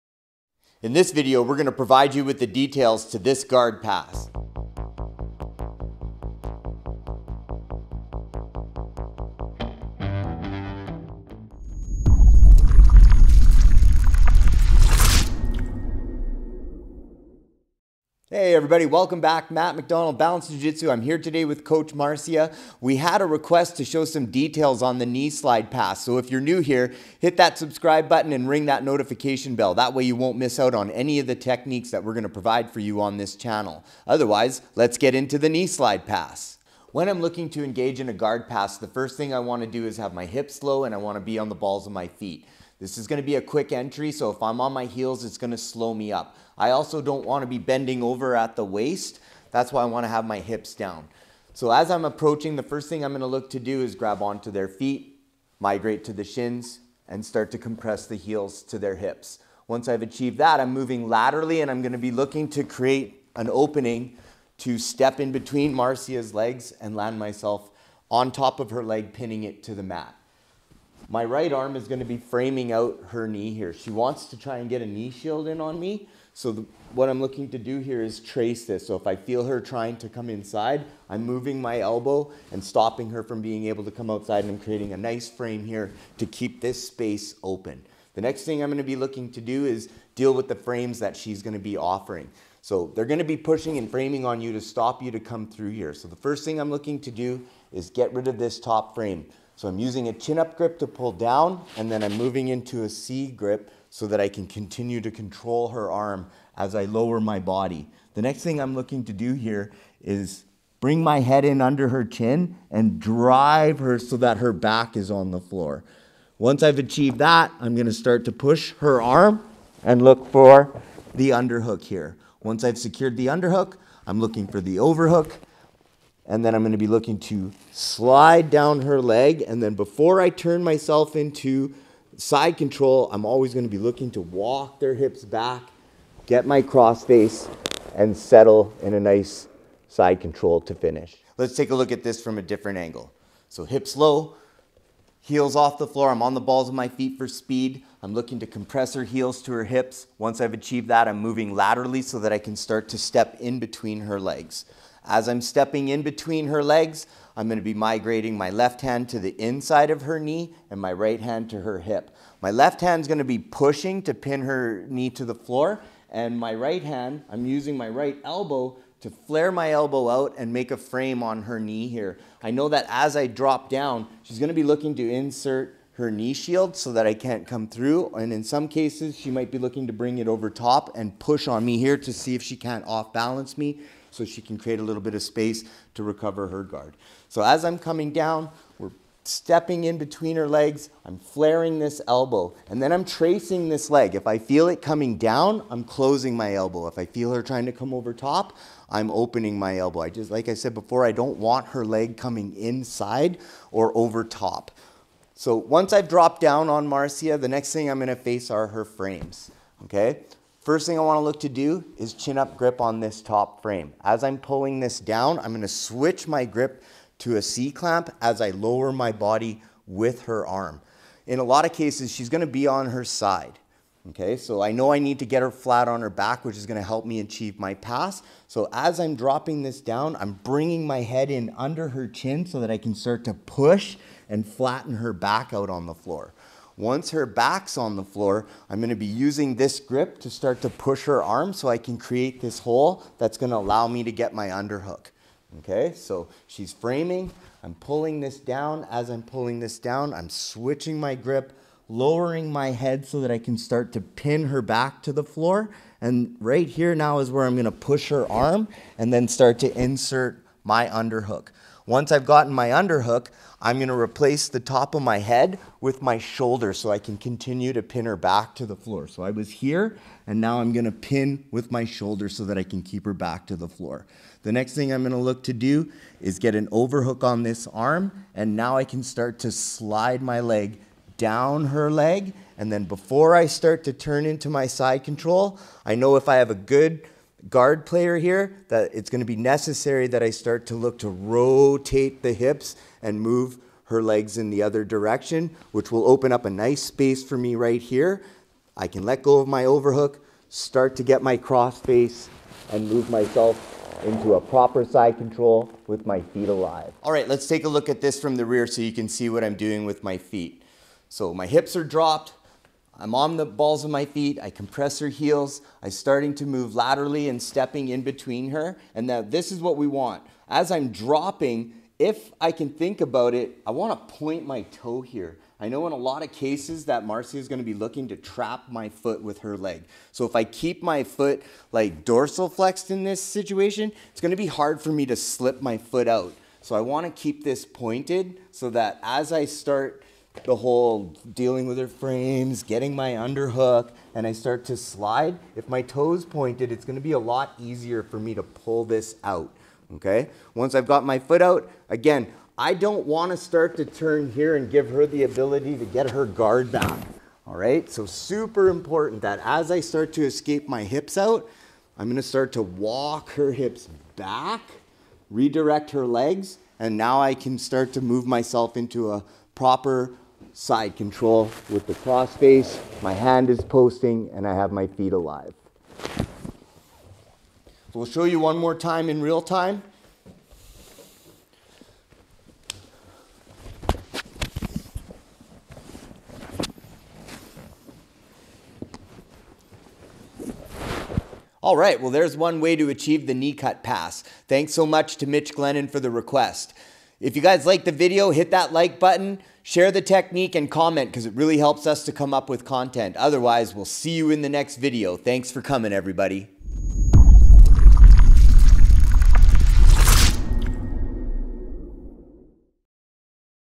How to do the Knee slide Pass/BJJ
Learn how to do the knee slide pass for bjj. In this video second degree black belt Professor Matt MacDonald walks you through the details on how to do the knee slide pass for Brazilian Jiu Jitsu. Understanding how to approach your opponent and enter with proper grips and frames will help you to up your percentage on finishing this popular stand up guard pass.